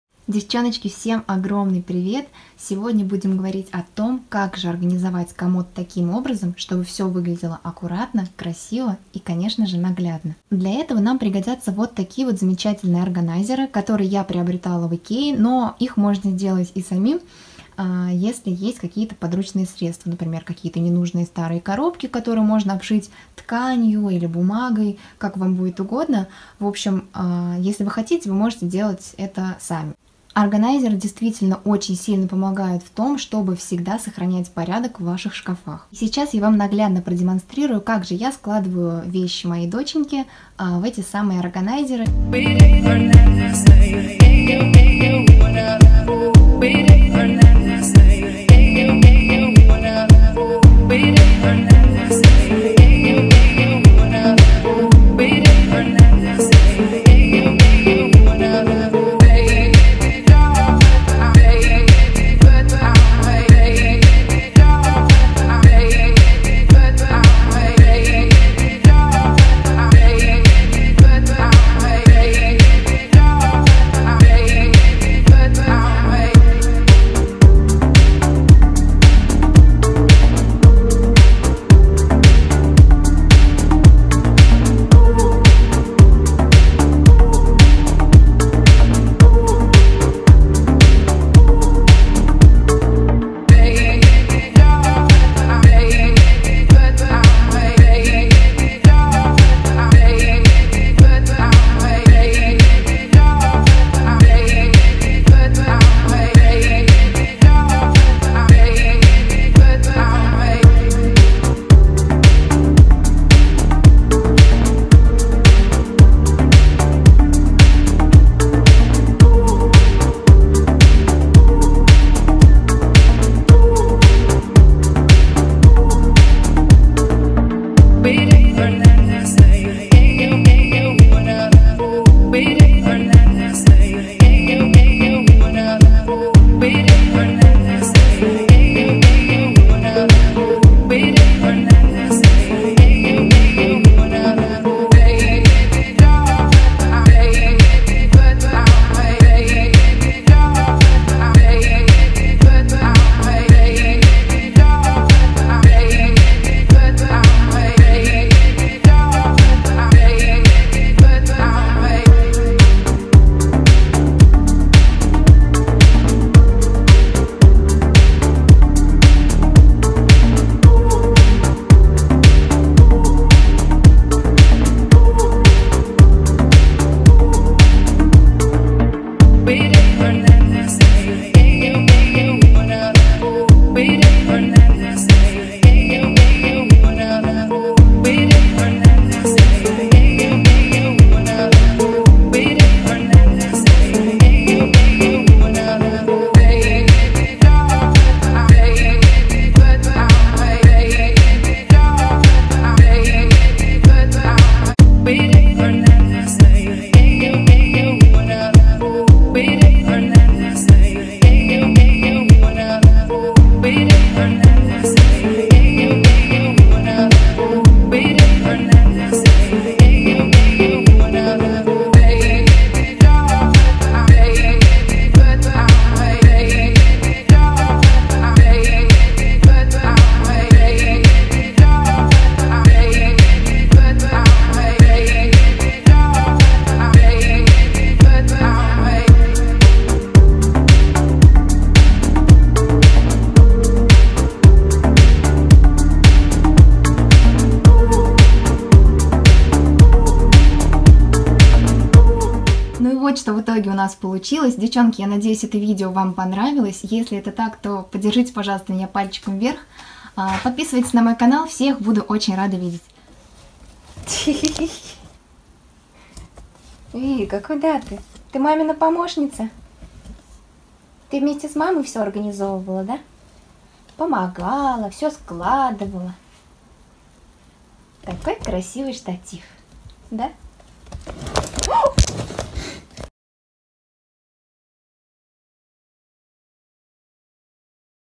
Как я складываю детскую одежду в комод? | Хранение и организация вещей | Taisia S
В этом видео показала как я складываю вещички моей доченьки в комод. Возможно для кого-то из Вас данное видео будет полезным и мотивирующим))

Если вы не видели как выглядел наш пеленальный комод во время его использования,то можете посмотреть вот это видео

ТОВАРЫ С AliExpress, КОТОРЫЕ Я РЕКОМЕНДУЮ:
ИГРУШКИ: